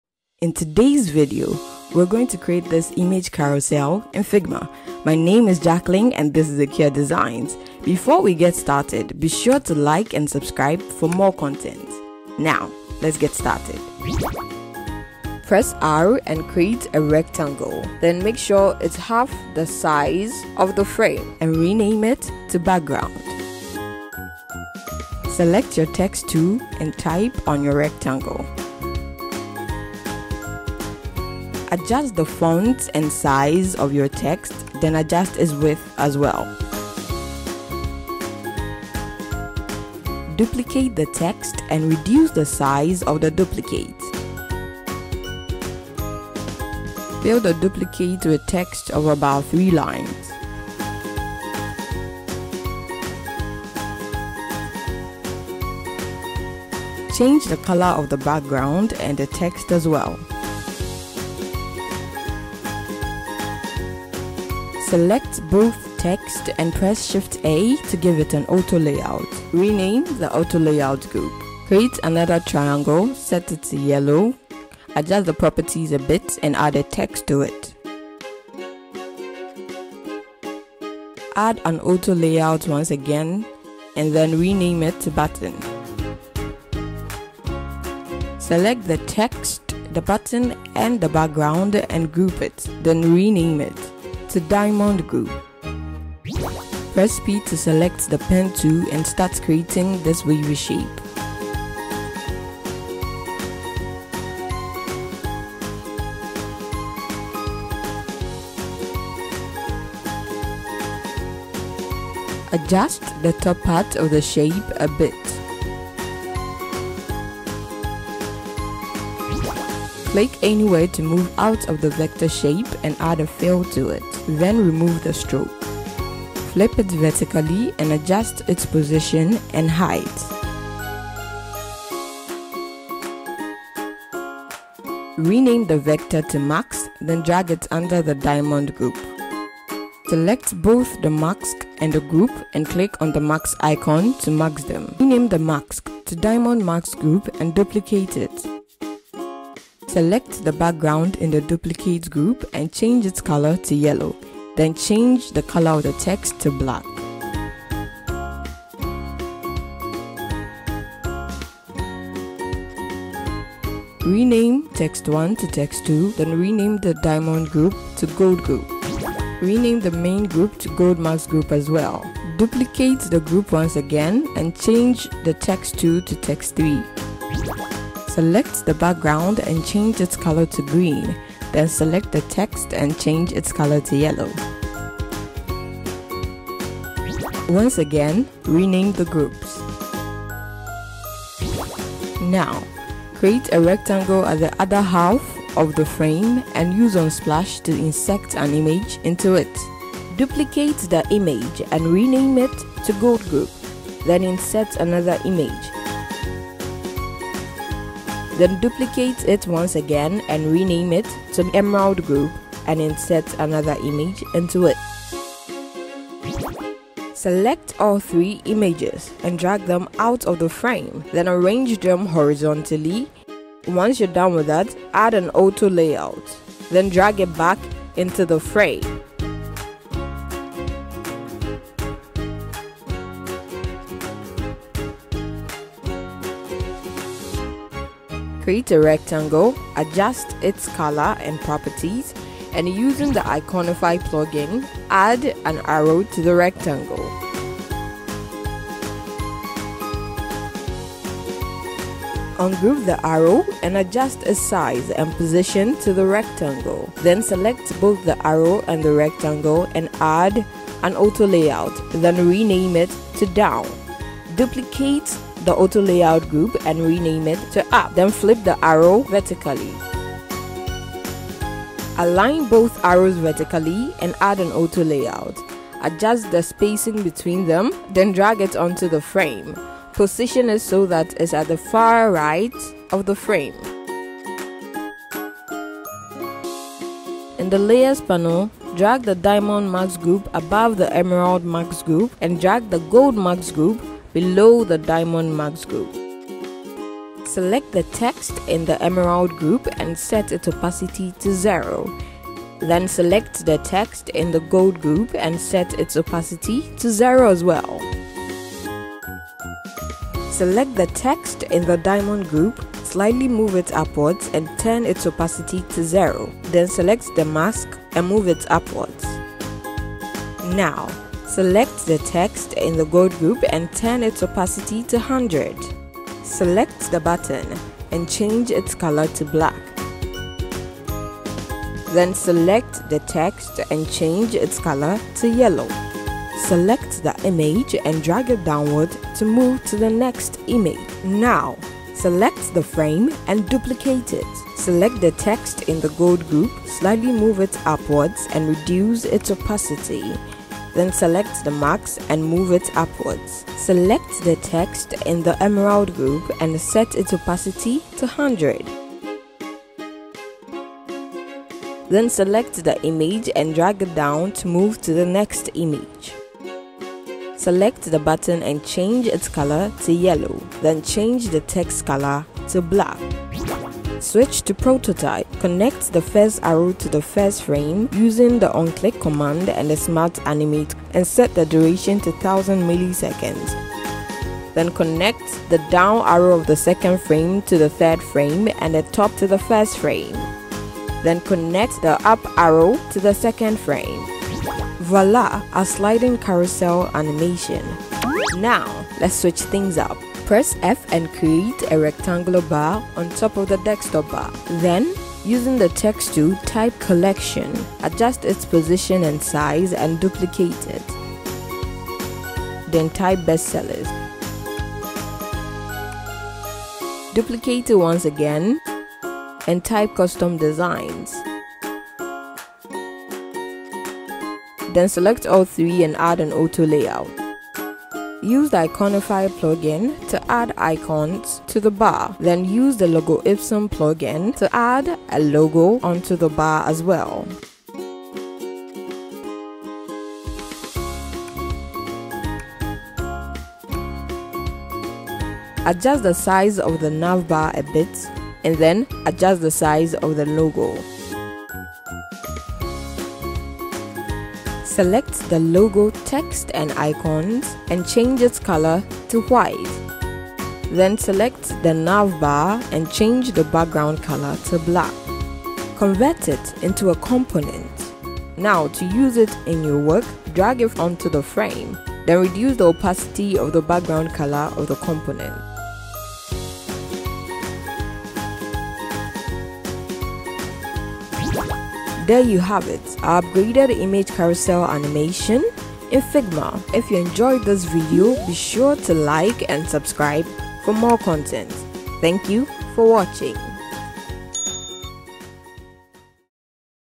How to Create an Image Carousel Animation in Figma: Step-by-Step Guide Tutorial
To learn about Figma Variables watch: "Designing A Mobile Dialpad With Figma Variables: A Quick And Easy Tutorial"
In this Figma tutorial, you'll learn how to create an image carousel animation from scratch. We'll walk you through the step-by-step process of designing and animating a carousel using Figma's powerful tools. By the end of this tutorial, you'll have a beautiful and functional carousel that you can use in your own projects. Whether you're a beginner or an experienced designer, this tutorial is perfect for anyone looking to improve their skills in Figma.
00:00 Introduction
00:16 Creating Carousel Text
03:16 Creating Carousel Images
06:37 1st Results
08:02 2nd Results